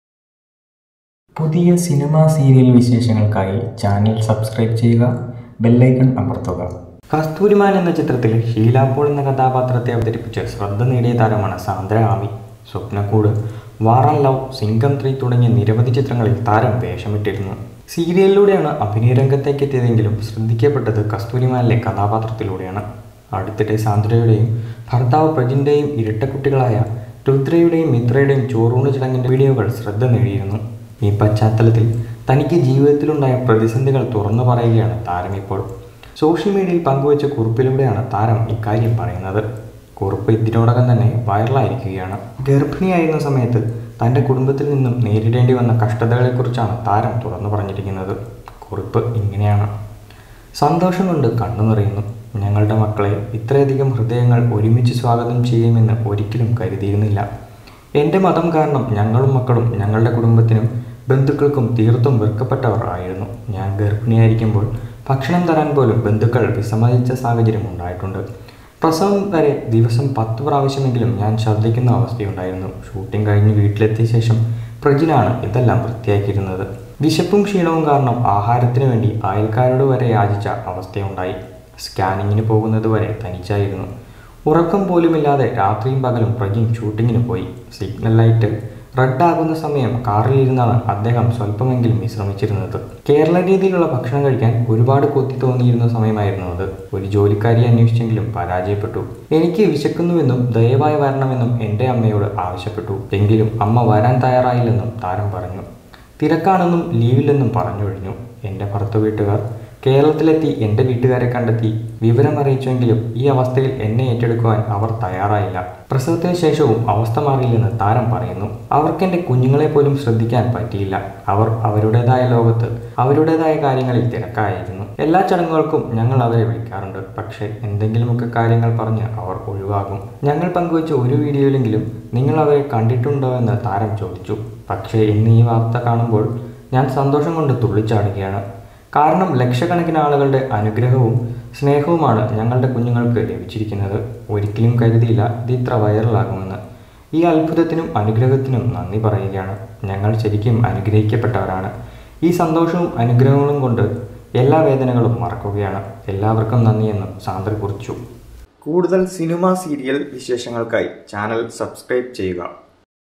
ഗര്‍ഭിണിയായിരുന്ന സമയത്ത് ഭക്ഷണത്തിനായി യാചിച്ചു'; കുഞ്ചാക്കോ ബോബന്റെ നായിക പറയുന്നു | sandra ami
'മതം കാരണം വെറുക്കപ്പെട്ടവരായി, ഗര്‍ഭിണിയായിരുന്ന സമയത്ത് ഭക്ഷണത്തിനായി യാചിച്ചു'; കുഞ്ചാക്കോ ബോബന്റെ നായിക പറയുന്നു